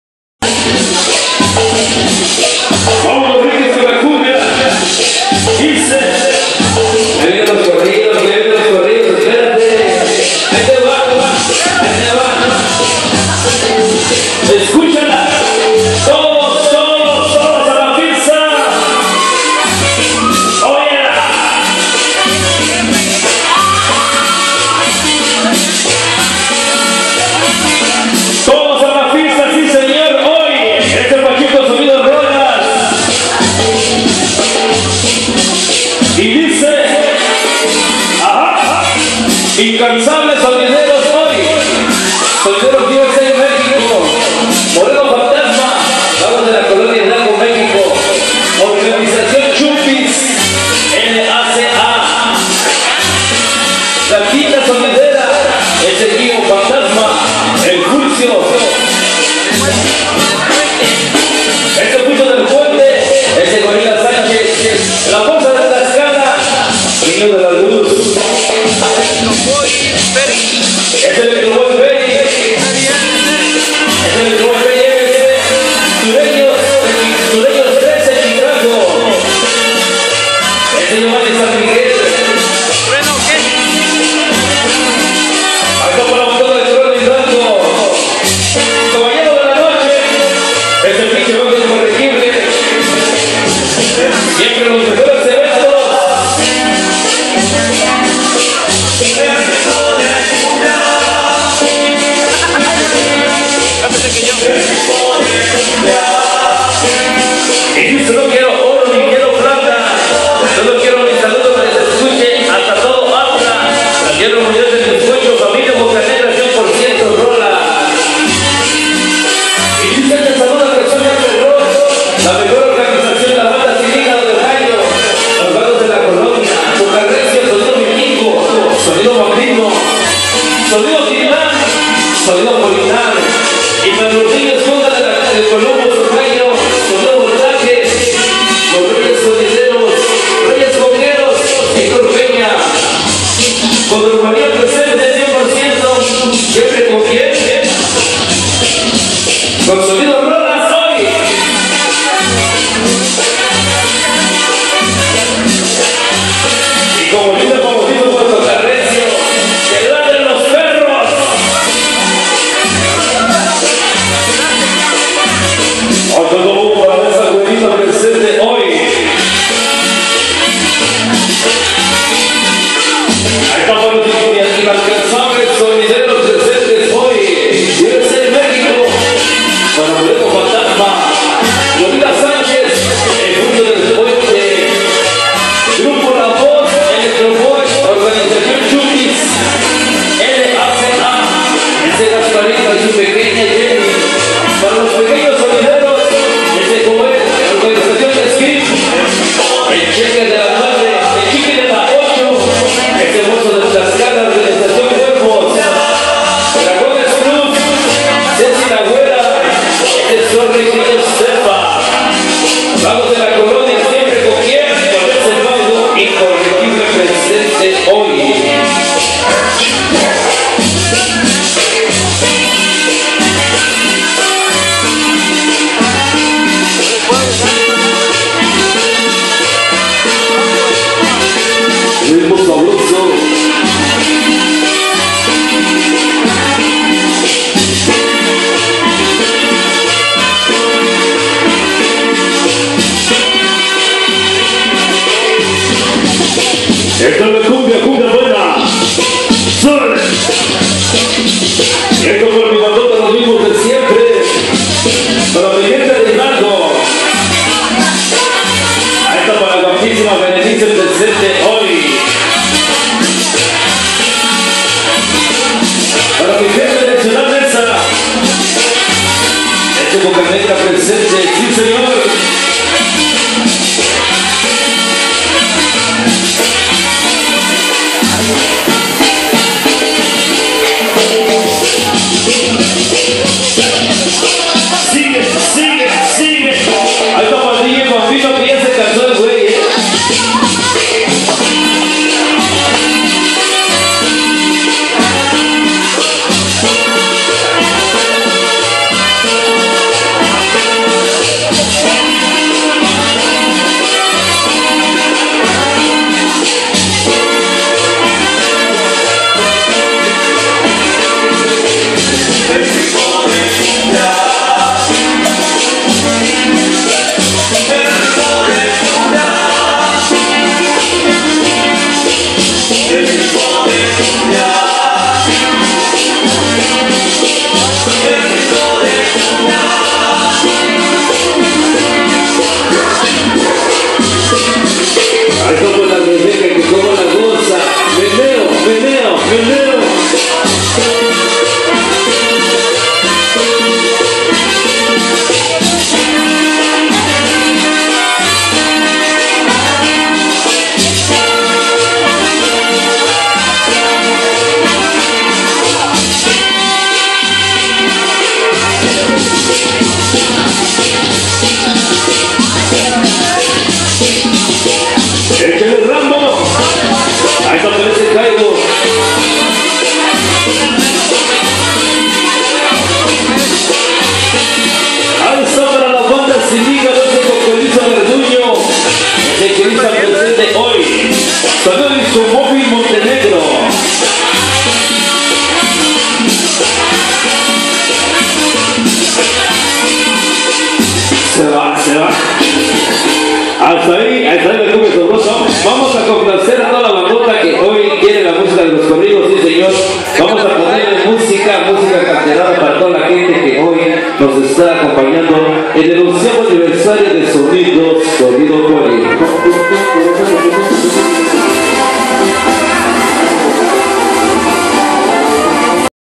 Embrujo de cumbia Sonido Rolas
Embrujo de Cumbia con el Pachuco ☆Sonido Rolas ☆ de columbus ohio U.S.A. en el 11 aniversario de sonido Acuario de Columbus ohio.